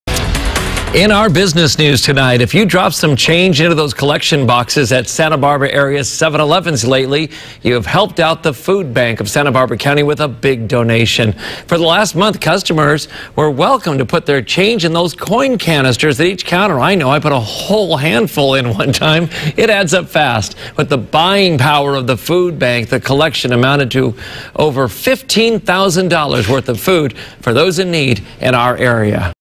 7-Eleven Helps Santa Barbara County Food Bank
Santa Barbara area 7-Eleven stores have united to raise money for the Food Bank of Santa Barbara County.